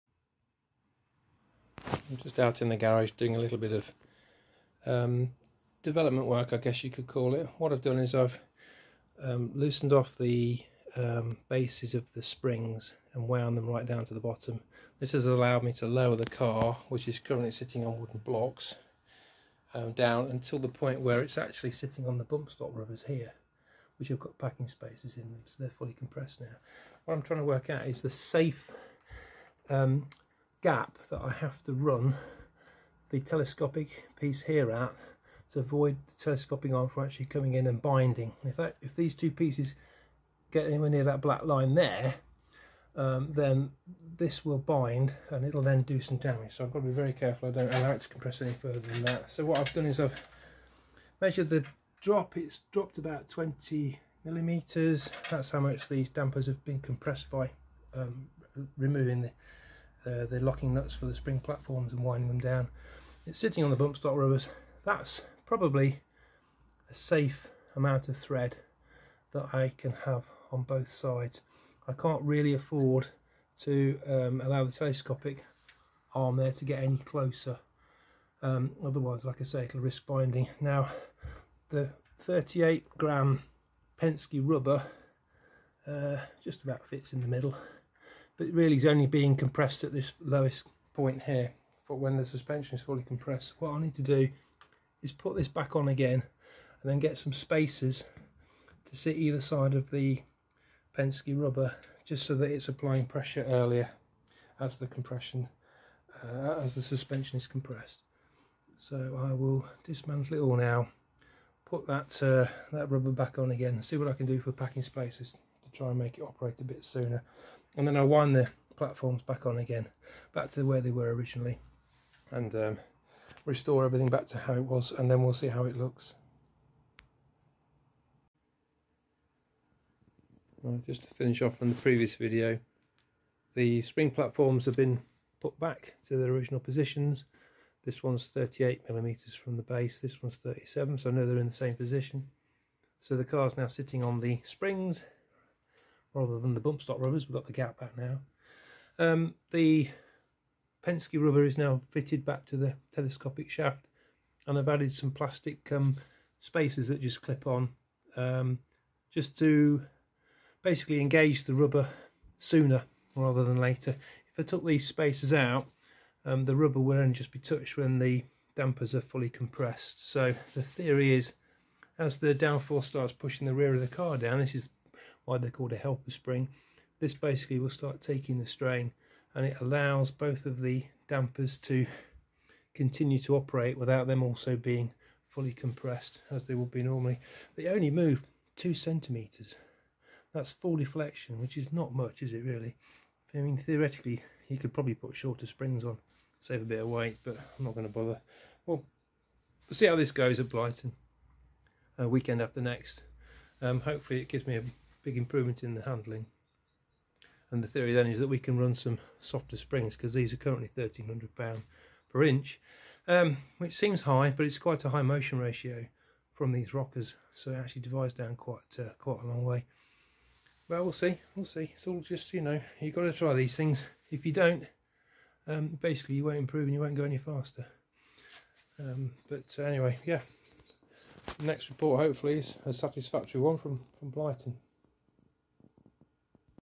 Setting up the self designed heave spring assembly on my Mygale EcoBoost
The length of the telescopic shaft needs to be setup to prevent the shaft from binding as it is compressed. This involves moving the spring platforms on the dampers to allow the suspension to be fully compressed, which shows the range of movement that the spring assembly has to allow.

I designed the parts myself in Fusion 360, and had them machined from 6061T6 aluminium.

Chapters
0:00 start
0:29 working out the safe working length of the telescopic spring
0:53 measuring the length
2:09 the rubber spring is fitted
3:27 running softer springs